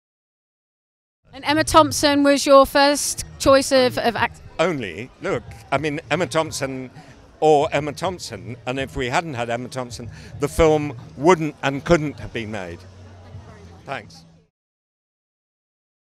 The Children Act - UK Premiere Interview - Richard Eyre
Childless, Judge Fiona Maye’s marriage is in trouble. Immersing herself in the safety of her work, she is presented with a case, that of 17 year old Jehovah’s Witness, Adam Henry.  Adam has Leukaemia and is declining a desperately needed blood transfusion. With The Children Act 1989 in mind Maye will need to decide whether to rule in favour of the medical profession who wish to save his life, or follow the wishes of a dying boy.

At the UK Premiere of The Children Act, Premiere Scene’s Claire Bueno and Anthony Bueno had the welfare of the film talent in mind when they interview actors Emma Thompson, Fionn Whitehead, Anthony Calf, director Richard Eyre, writer of book and screenplay Ian McEwan and producer Duncan Kenworthy.

Join us as we ask Richard Eyre on Emma Thompson being the only gal for the job!